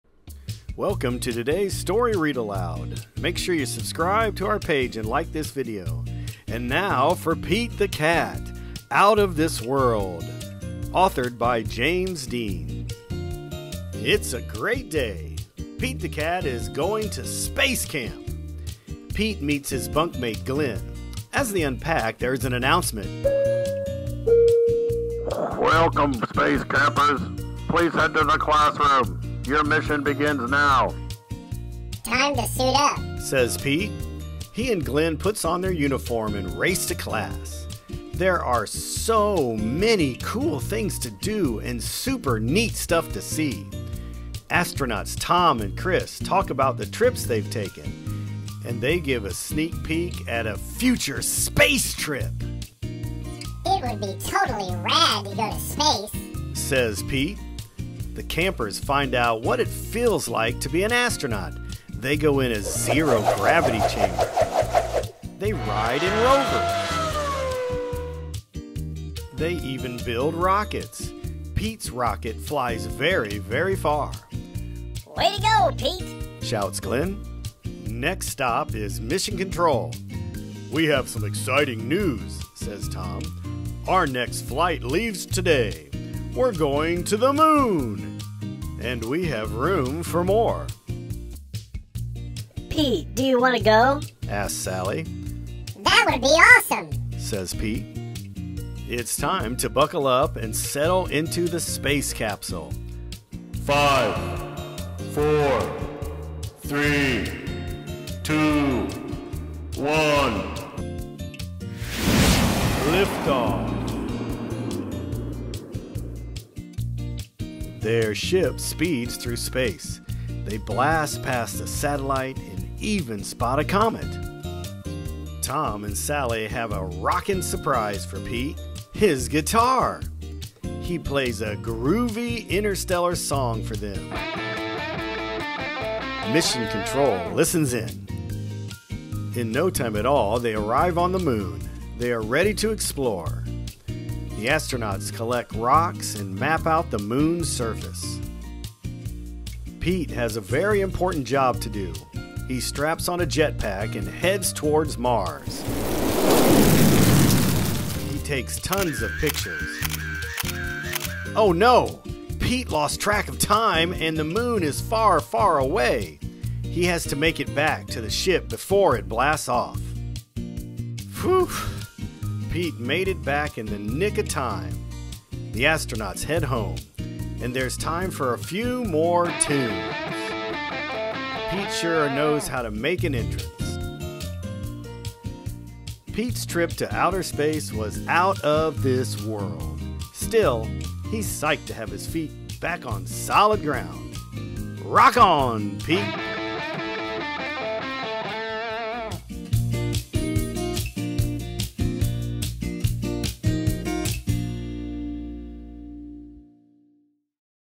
Pete the Cat Out of This World - A Read Aloud Story for Kids of All Ages
Blast off into space with New York Times bestselling author and artists James Dean and everyone’s favorite cat-stronaut, Pete the Cat, in Pete the Cat: Out of This World.

When Pete heads off to space camp, he gets to float in zero gravity, build rockets, and even goes on an extraordinary interstellar mission that is out of this world—literally. Houston, ready to rock out with Pete the Cat!

AR Quiz #192139